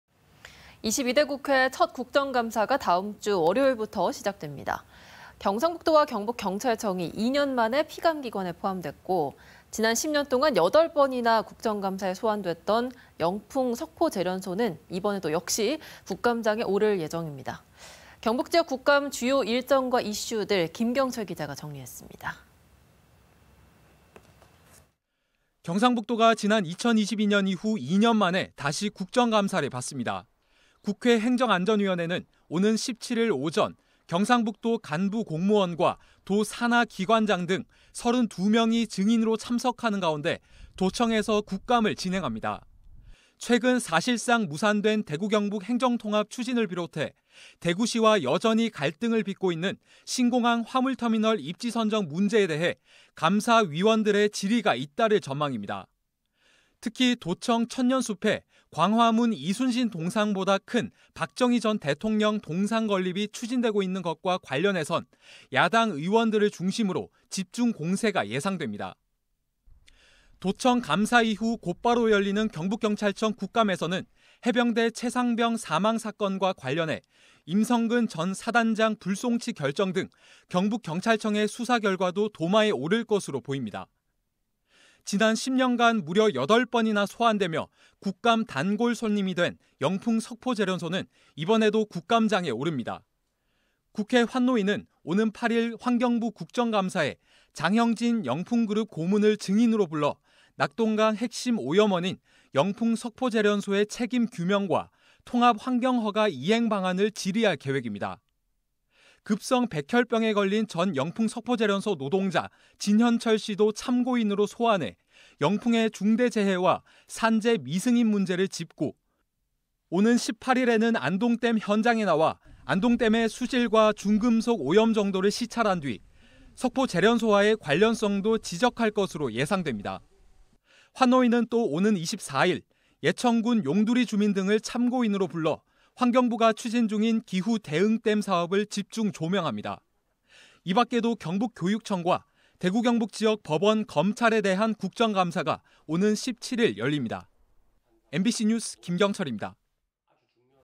경북 국정감사, '신공항·안동댐·영풍 석포제련소' 주목 | 대구MBC뉴스
내용: 경북 국정감사, '신공항·안동댐·영풍 석포제련소' 주목 - 김경철 기자 (안동M)